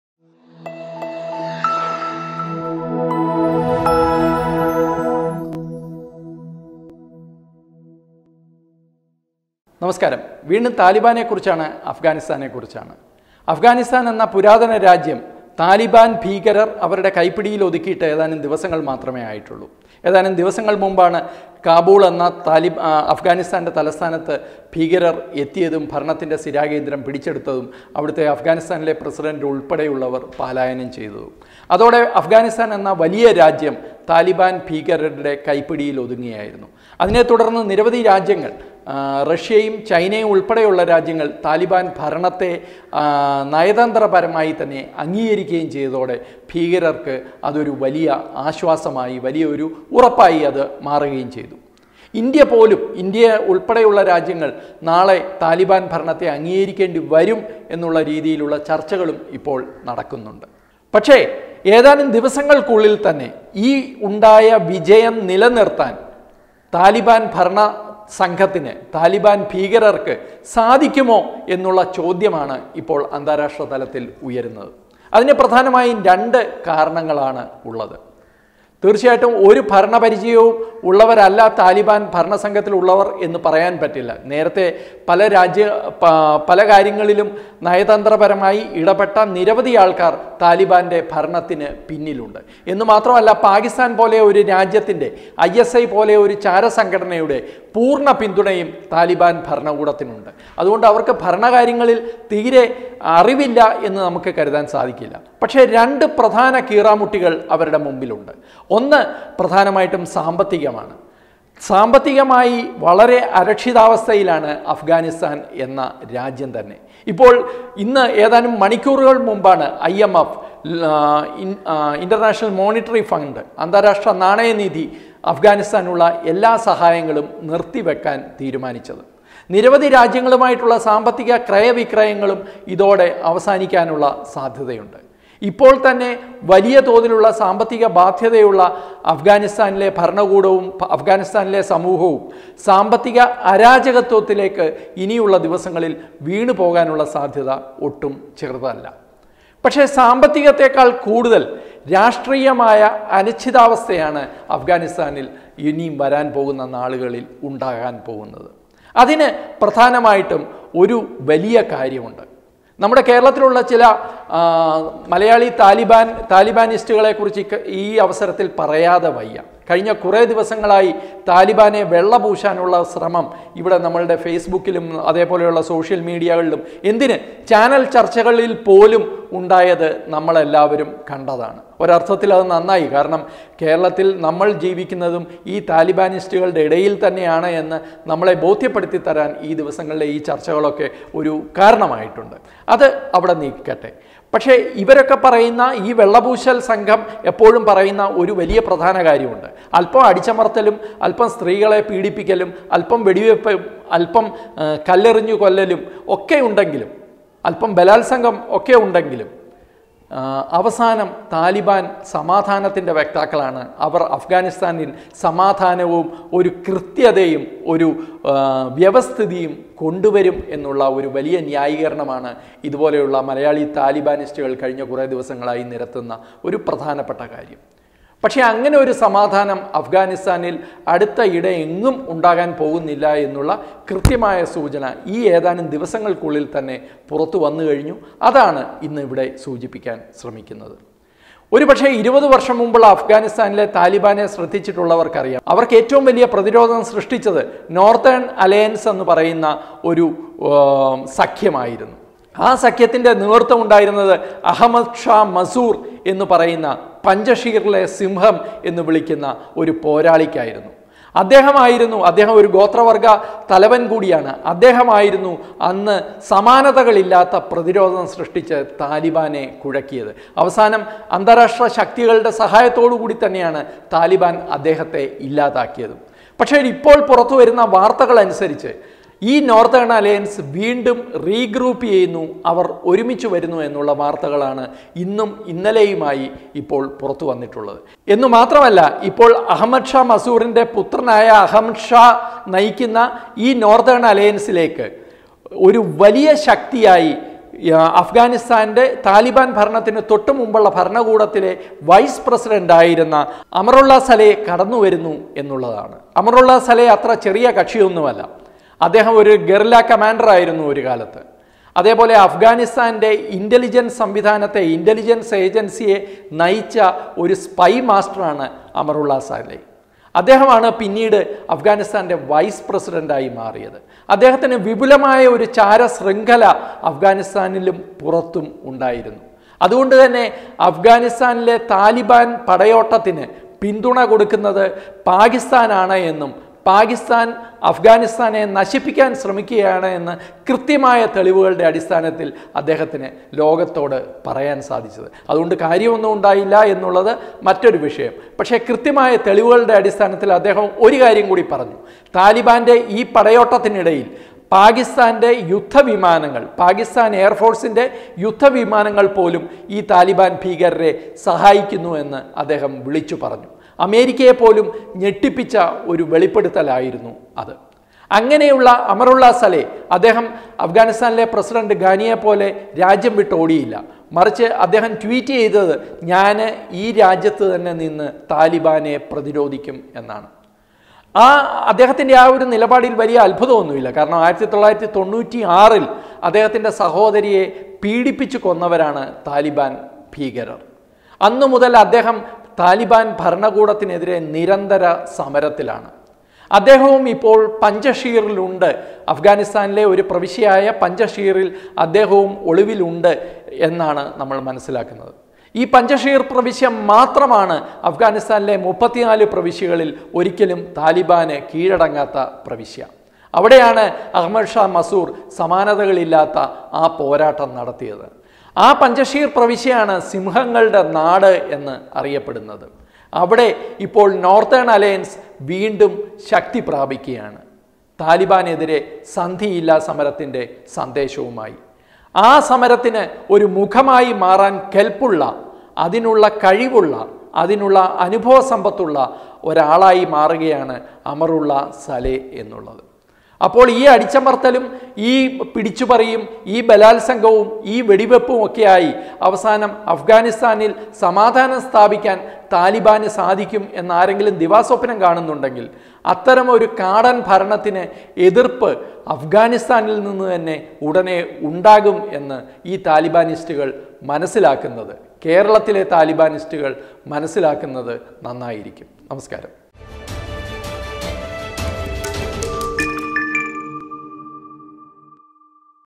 കാടൻ ഭരണത്തിൽ നിന്ന് അഫ്ഗാനെ മോചിപ്പിക്കാൻ പുതിയ സഖ്യം?| RP THOUGHTS
മലയാളിതാലിബാനിസ്റ്റുകൾ അധികം അർമാദിക്കേണ്ട.. അഫ്‌ഗാനിസ്ഥാനിൽ ഉയരുന്നത് വൻപ്രതിഷേധം | RP THOUGHTS